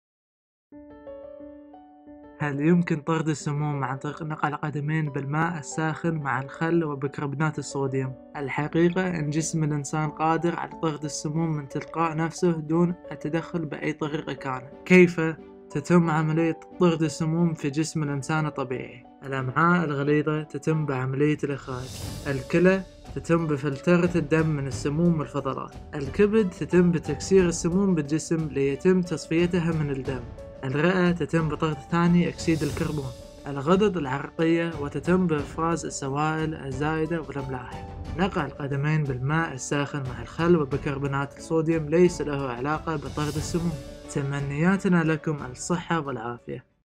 هل يمكن طرد السموم عن طريق نقع القدمين بالماء الساخن مع الخل وبيكربونات الصوديوم؟
الحقيقة أن جسم الإنسان قادر على طرد السموم من تلقاء نفسه دون التدخل بأي طريقة كانت

كيف .. تتم عملية طرد السموم في جسم الإنسان الطبيعي؟

الكلى .. تتم بفلترة الدم من السموم والفضلات

الأمعاء الغليضة تتم بعميلة الإخراج

الغدد العرّقية وتتم بإفراز السوائل الزائدة والأملاح

الكبد تتم بتكسير السموم بالجسم ليتم تصفيتها من الدم

الرئة تتم بطرد ثاني اكسيد الكربون

نقع القدمين بالماء الساخن مع الخل وبيكربونات الصوديوم ليس له علاقة بطرد السموم

تمنيتنا لكم الصحة والعافية